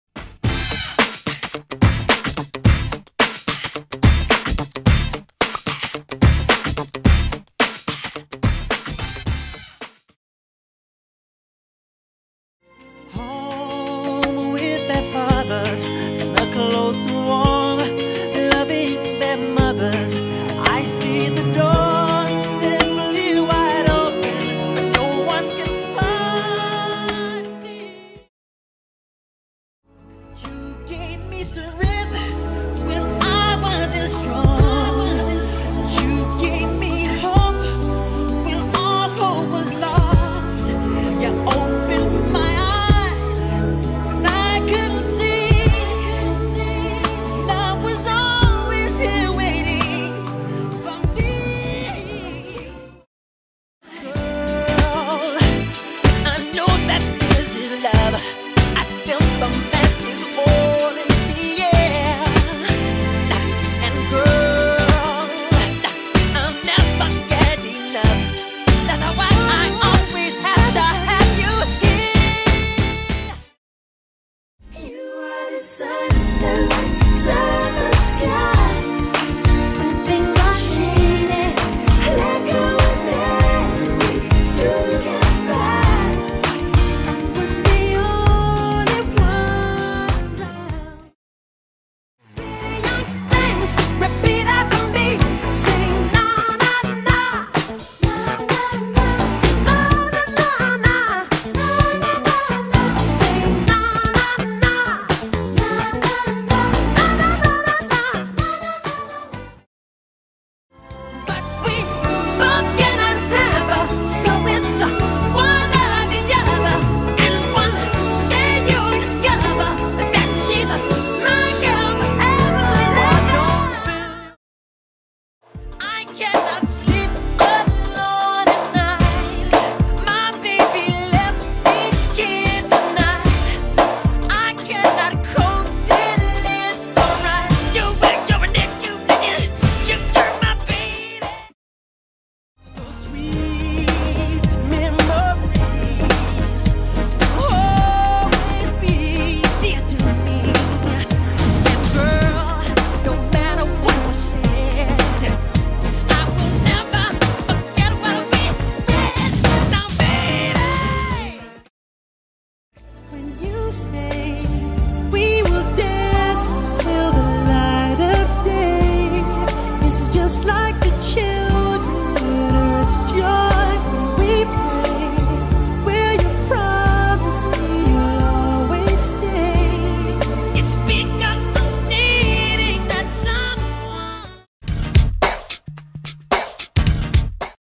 Michael Jackson’s Best Bridges
Here is a compilation of amazing bridges in Michael Jackson songs. Inspired by a dear friend of mine.